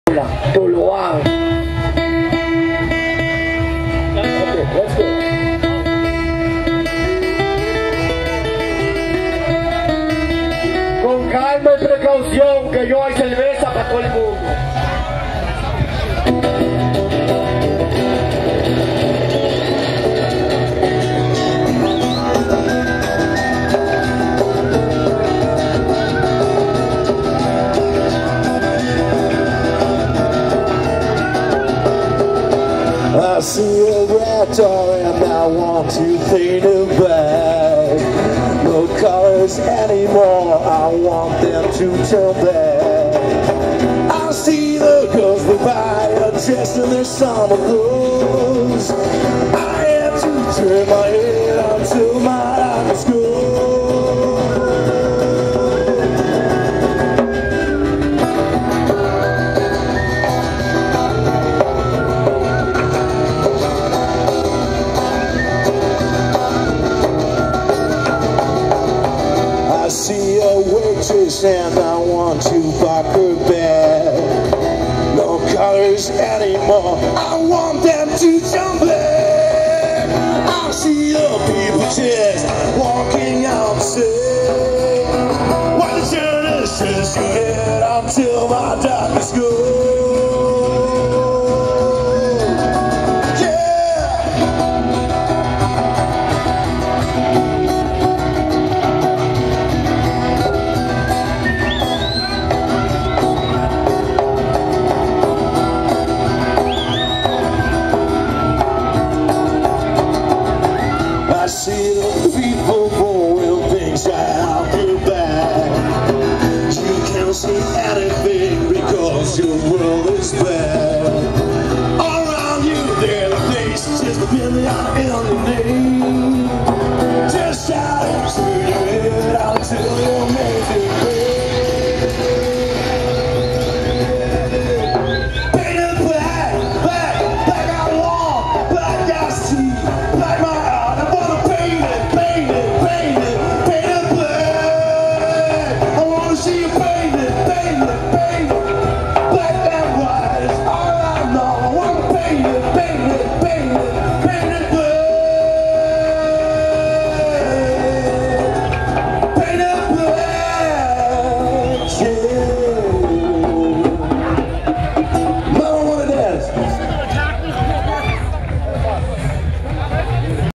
Mama Wanna Dance during Easter 2011 on Cabarete Playa
Performance of Mama Wanna Dance on the Easter Festival in Cabarete in the Dominican Republic by Musicimpassioned.com